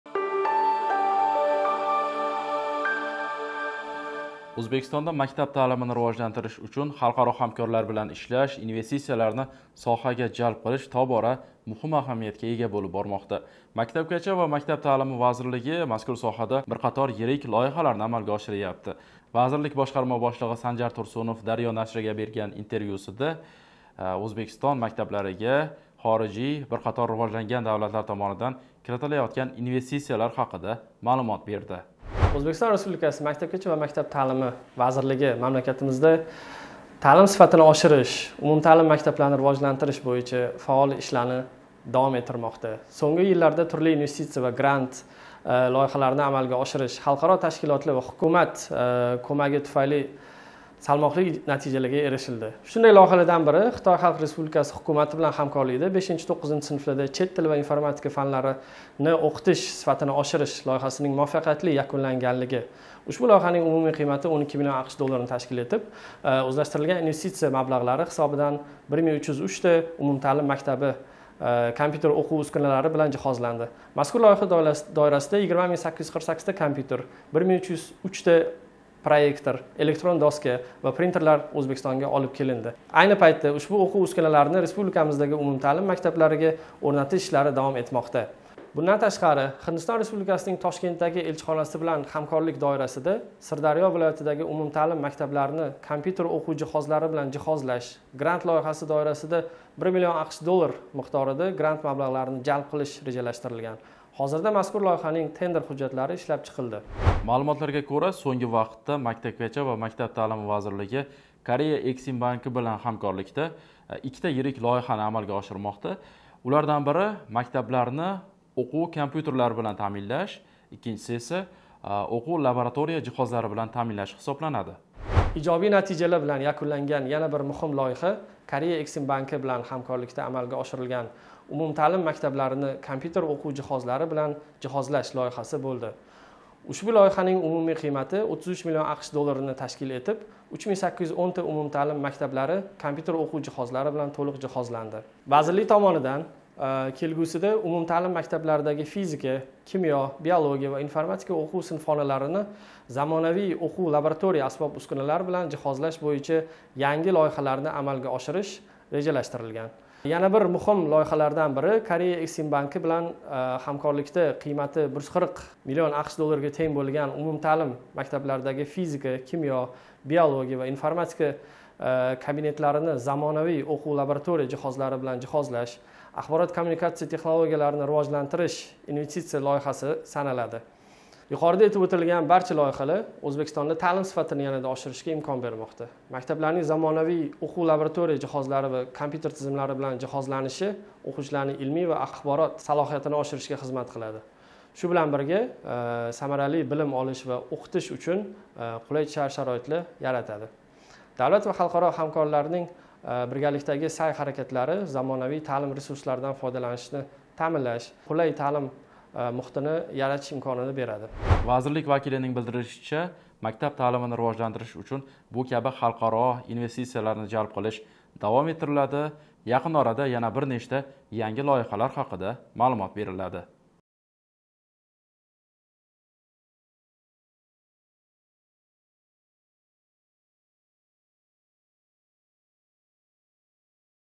Oʻzbekiston maktablariga Koreya 140 mln, Xitoy 12 mln dollar sarmoya kiritdi
Maktabgacha va maktab taʼlimi vazirligi tomonidan xalqaro aloqalarni kengaytirish va investitsiyalarni jalb qilish boʻyicha qator loyihalar amalga oshirilmoqda. Xususan, Xitoy hukumati bilan hamkorlikda Oʻzbekiston maktablarini kompyuter oʻquv uskunalari bilan jihozlash loyihasi yakunlandi. Vazirlik boshqarma boshligʻi Sanjar Tursunov “Daryo” nashriga bergan intervyusida mazkur hamkorlik toʻgʻrisida maʼlumot berdi.

“Xitoy bilan hamkorlikda amalga oshirilgan “5-9-sinflarda chet tili va informatika fanlarini oʻqitish sifatini oshirish" loyihasining umumiy qiymati 12 million dollarini tashkil etdi. Oʻzlashtirilgan investitsiya mablagʻlari hisobidan 1 303 ta umumtaʼlim maktabi kompyuter oʻquv uskunalari bilan jihozlandi. Mazkur loyiha doirasida 20 848 ta kompyuter, 1 303 ta proyektor, elektron doska va printerlar Oʻzbekistonga olib kelindi. Ayni paytda ushbu oʻquv uskunalarini respublikamizdagi umumtaʼlim maktablariga oʻrnatish ishlari davom etmoqda”, — dedi vazirkli vakili.

Shu bilan birga, vazirlik Koreya Ekzimbanki bilan hamkorlikda 2 ta yirik loyihani amalga oshirmoqda.

“Birinchisi, “Umumtaʼlim maktablarini kompyuter oʻquv jihozlari bilan jihozlash” loyihasi boʻlib, uning umumiy qiymati 33 million dollarni tshakil etadi. Mazkur loyiha doirasida Oʻzbekistondagi 3 810 ta maktab kompyuter oʻquv jihozlari bilan toʻliq jihozlandi. Koreya Eksimbanki bilan hamkorlikda amalga oshirilgan yana biri yirik loyiha, “Umumtaʼlim maktablaridagi fizika, kimyo, biologiya va informatika kabinetlarini zamonaviy oʻquv-laboratoriya jihozlari bilan jihozlash, axborot-kommunikatsiya texnologiyalarini rivojlantirish” investitsiya loyihasi hisoblanadi. Uning umumiy qiymati — qiymati 140 million dollarga teng”, — dedi mutaxassis.